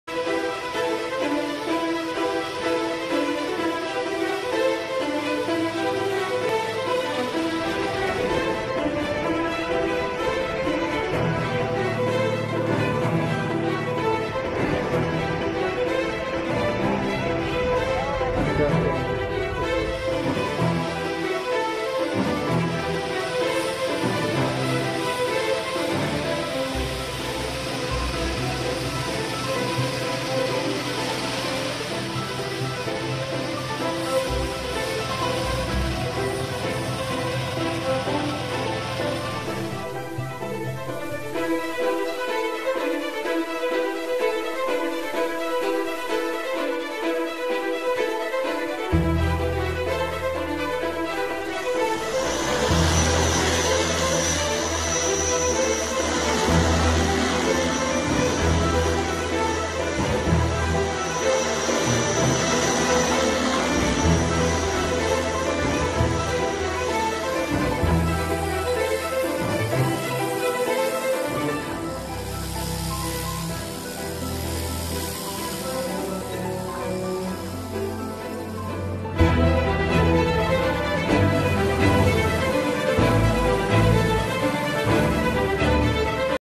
Coronavirus Update: China is disinfecting entire city today
China is continuously disinfecting and destroying corona virus entire city through giant truck.